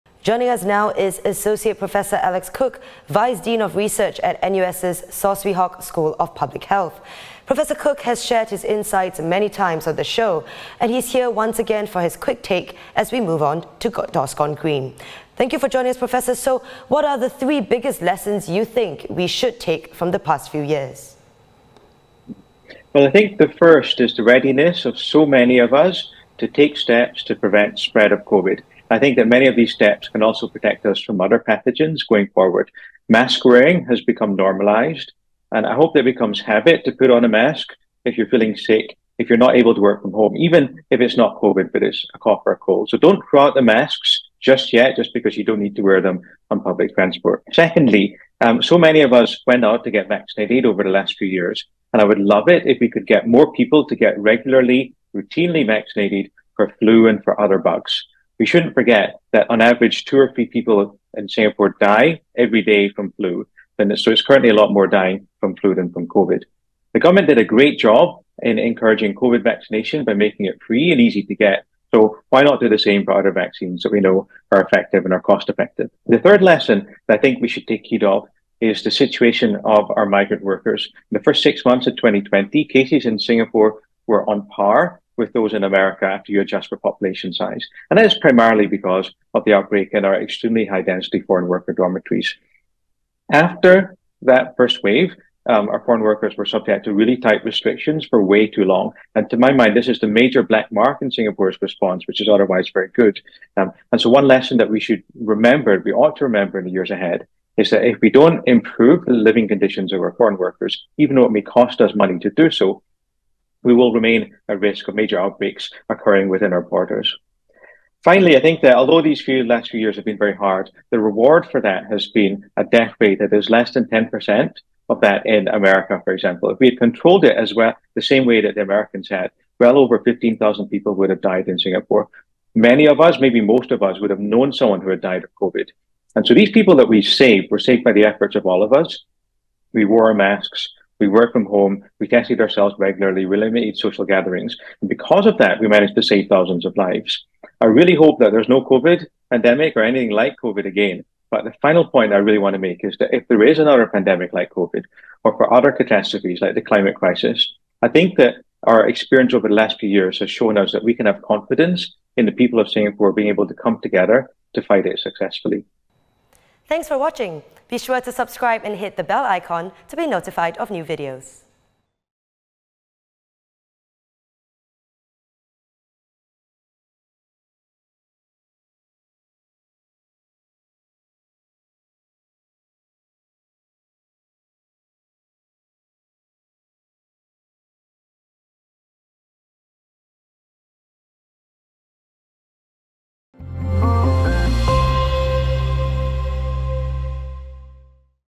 'Don't throw out masks': Expert hopes mask-wearing becomes habit when people are ill | THE BIG STORY
As Singapore looks set to move to Dorscon green on February 13, Associate Professor Alex Cook, vice-dean of research at NUS' Saw Swee Hock School of Public Health, gives his take on the top lessons we should learn from our battle with Covid-19. He hopes mask-wearing will become habit whenever a person falls ill.